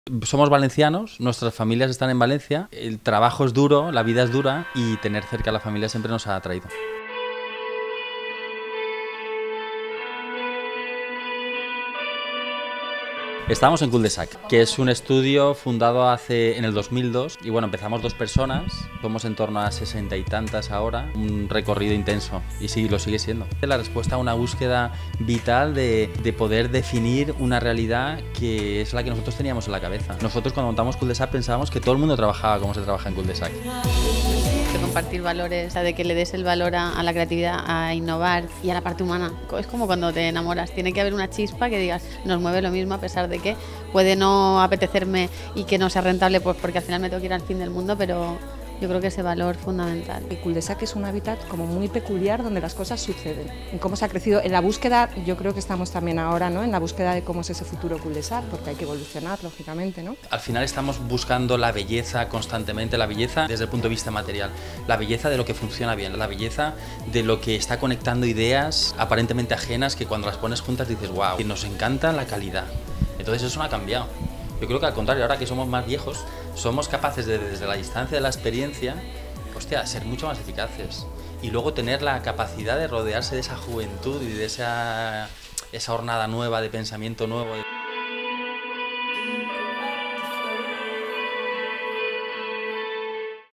Cul de Sac, con amor desde Valencia #974 · Tendencias.tv
Cul de Sac nace de una actitud ante los retos y como respuesta a una búsqueda vital que no encontraban en otras empresas.